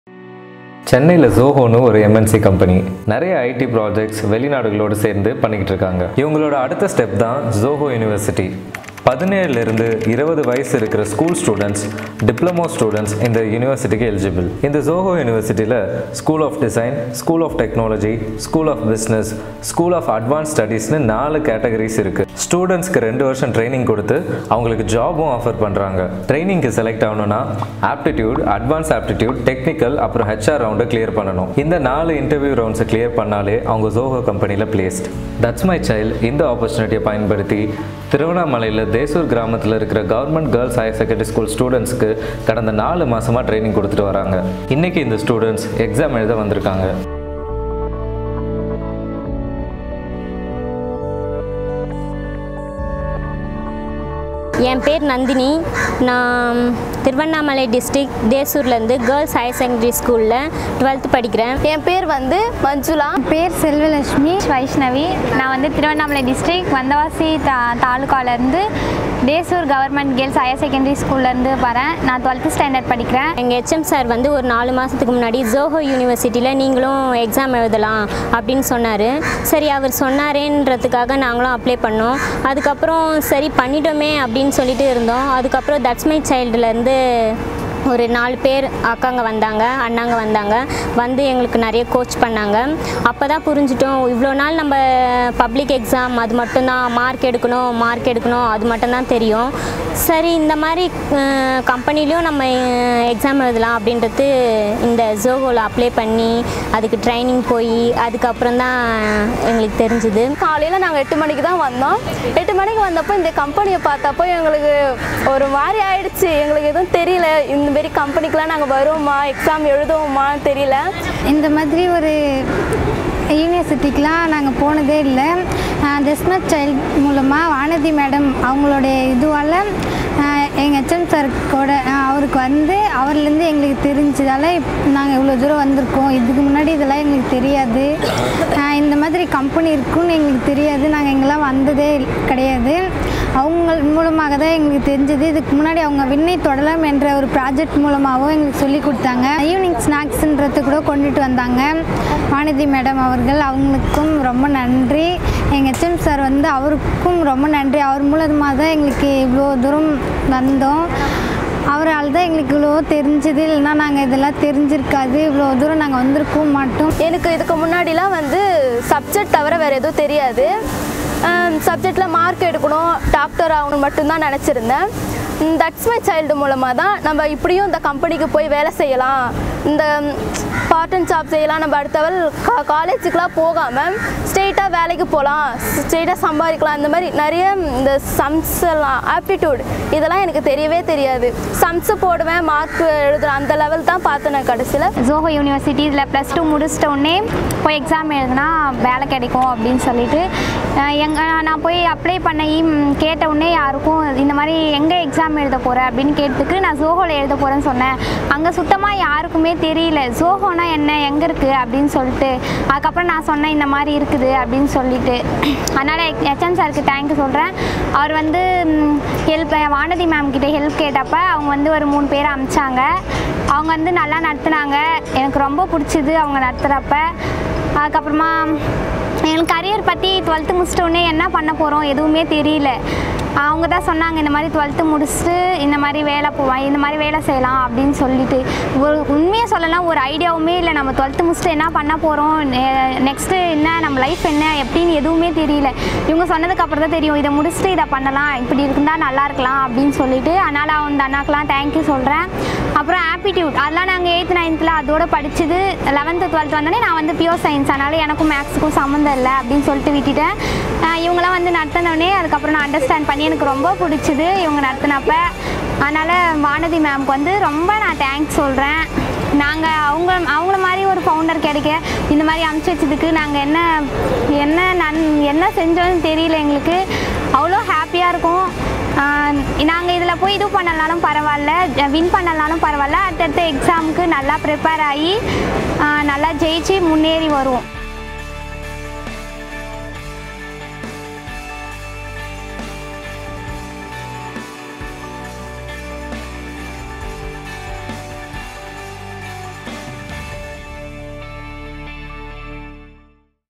ZOHO UNIVERSITY - A MEANINGFUL ALTERNATIVE TO CONVENTIONAL COLLEGE EDUCATION || ThatsMyChild
Zoho university's next recruitment for classes begins in May 2021.

Educate to Liberate the Underprivileged Children